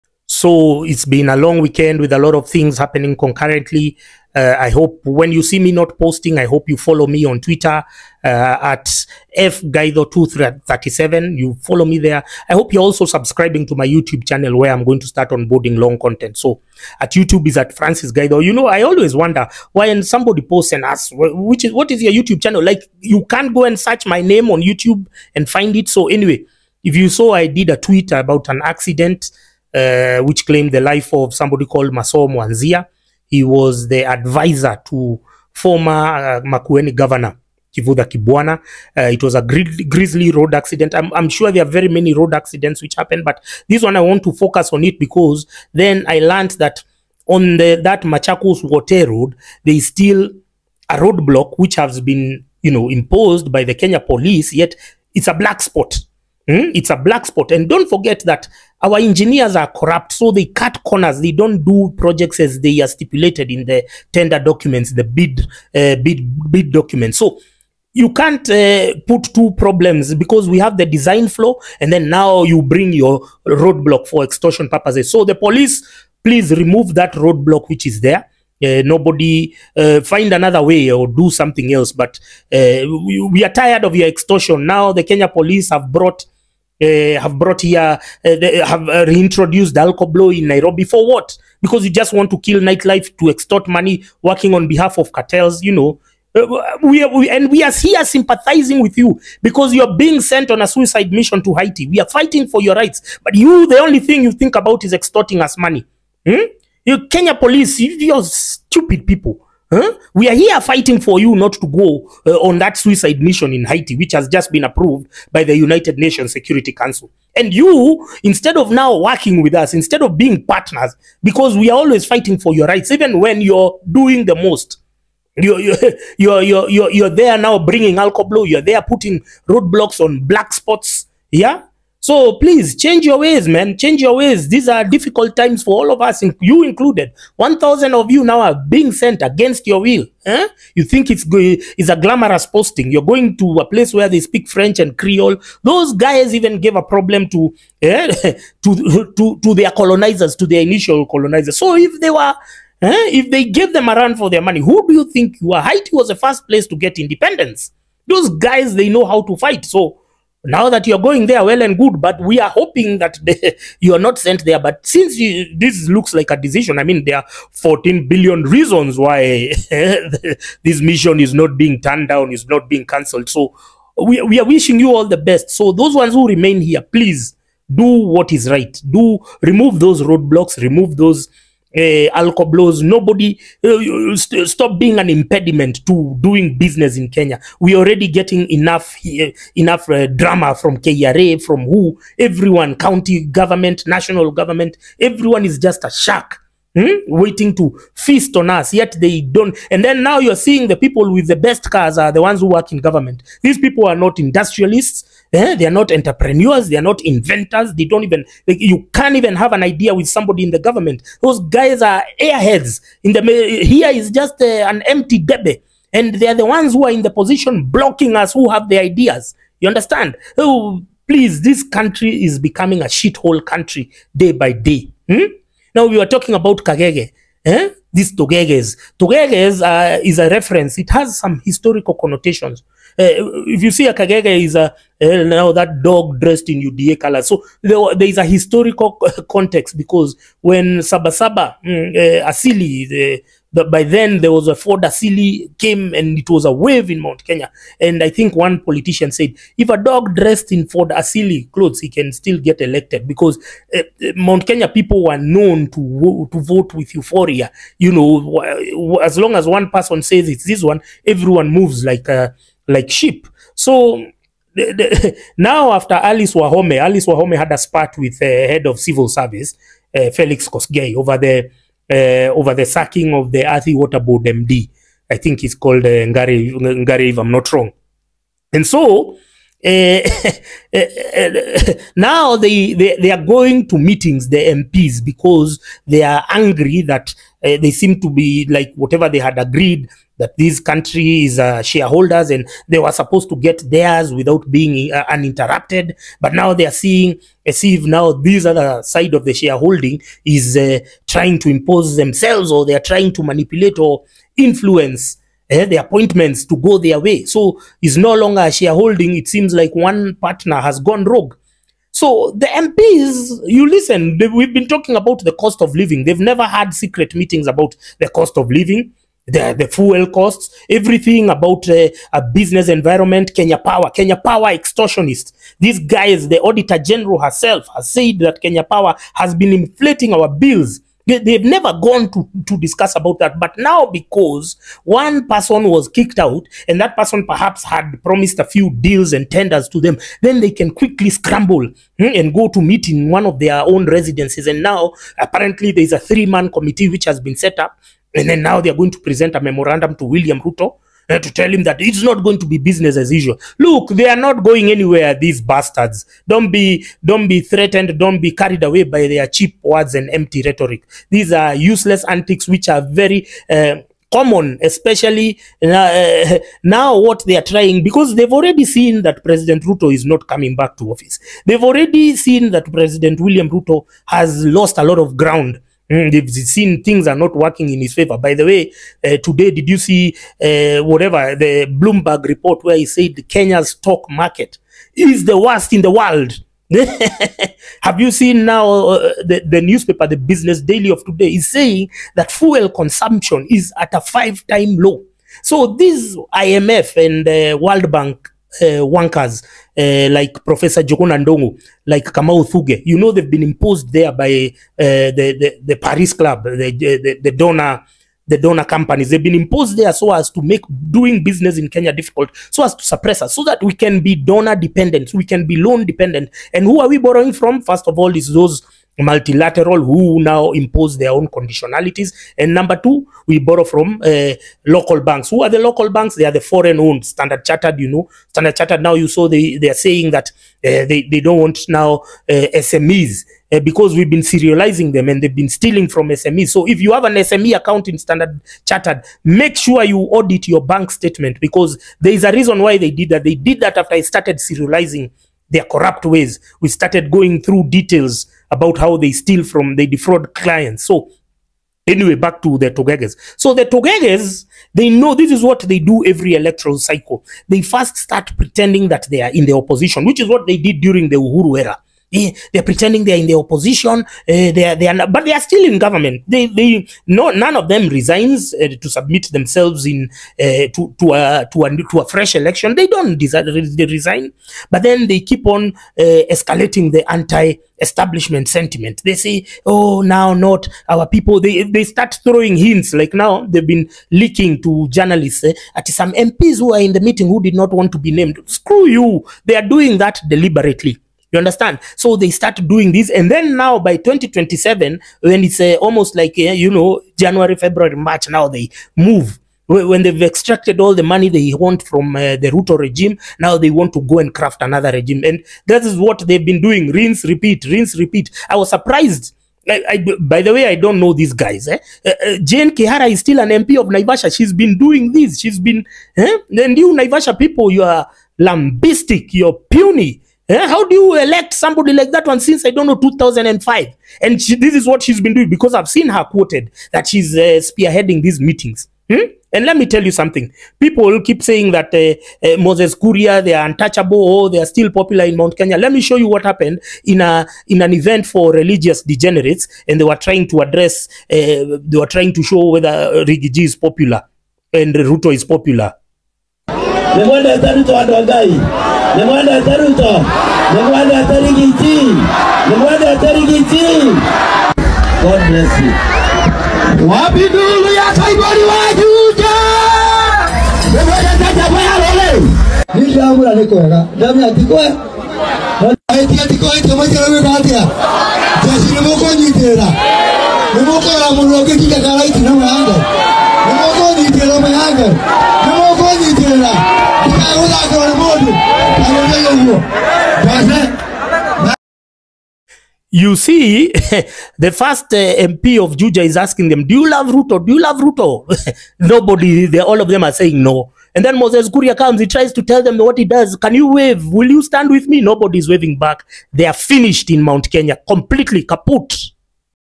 TUGEGE: Mount Kenya MPs Plot To Assert Dominance After Alice Wahome Drama
The term tugege or kagege has come ro define the extreme sycophants of the udaparty . And now there seems to be cracks from within after Water CS alicewahome locked horns with Head of Civil Service felixkoskei . A night meeting was held at riggyg Karen home while another was held at sabinachege Thika Greens home.